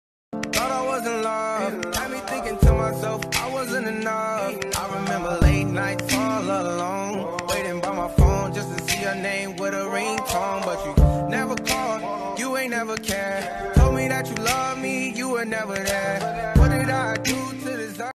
New Roblox edit :D
So guys I finnaly got robux!
And this my first edit with my new avatar.. :)